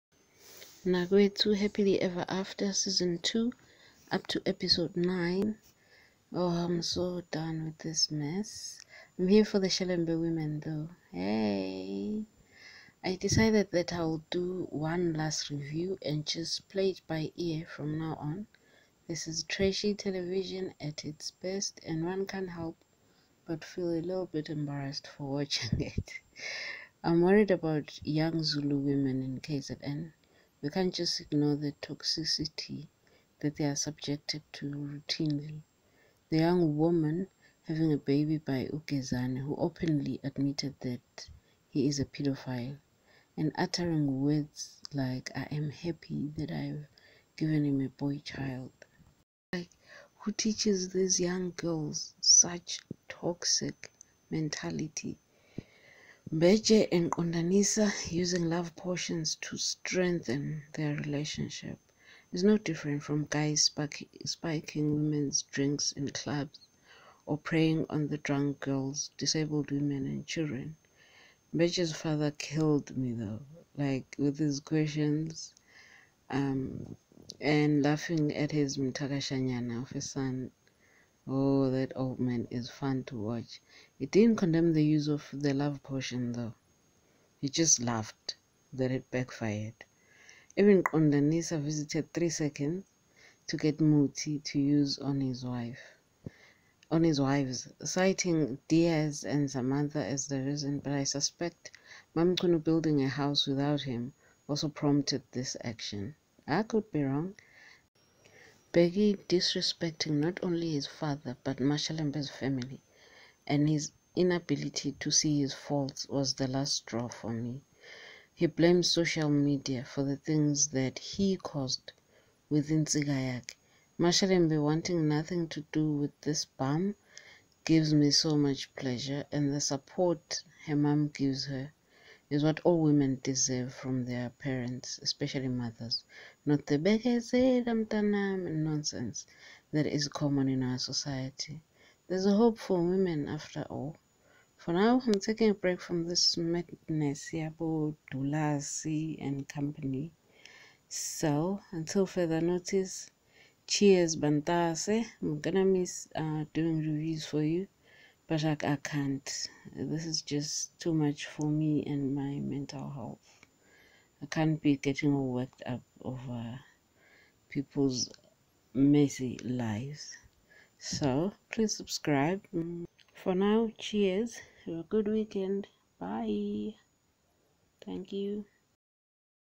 Mnakwethu Happily Ever After Season 2 up to Episode 9| Witchcraft
Mnakwethu Happily Ever After Season 2 up to episode 8. I'm here for the Shelembe women🙌!

I decided that I will do this last review and just play it by ear from now on. This is trashy television at its best and one can't help but feel embarrassed for watching it.

I am worried about young Zulu women in KZN. We can't just ignore the toxicity that they are subjected to routinely. The young woman having a baby by Gezane (who openly admitted that he is a peadophile) and uttering the words "I am happy that I have borne him a boy child..."

Mbeje and Qondanisa using love potions to "strengthen" their relationships is no different from guys spiking women's drinks in clubs or preying on drunk girls, disabled women and children!

Mbeje's father killed me with his questions to his mthakashana of a son. Oh that old man is fun to watch. He didn't condemn the use of the love potion though.

Even Qondanisa visited 3 seconds to get muthi to use on his wives, citing Diaz and Samantha as the reason but I suspect MaMchunu building a house without him also prompted this action. He can see that he's slowly losing her. Women, you are on your own.

Bheki disrespecting not only his father but MaShelembe's family and his inability to see his faults was the last straw for me. He blames social media for the things that he caused with his nsika. MaShelembe wanting nothing to do with this bum gives me pleasure and the support her mom gives her is what all women deserve from their mothers, not the 'bekezela mntanami' nonsense that is so common in our society. There's hope for women folk after all.

For now, I am taking a break from this madness yabo Dulas and company.

Cheers bangane🥂